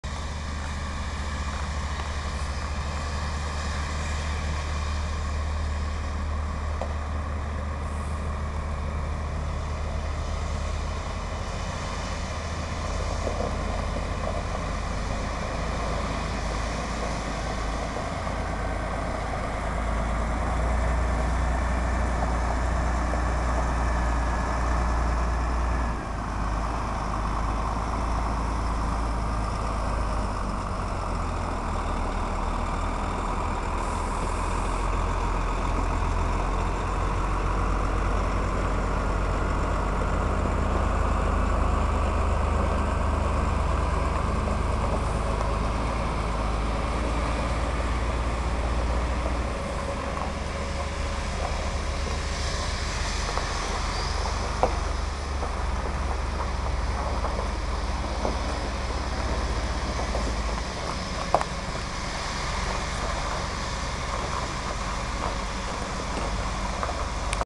Broshuis TCT taking Corner 02
Testing Tower Cup Trailer Ter Linden at Vestas. Cornering loaded with 24 meter, diameter from 3.820mm and 54 ton tower.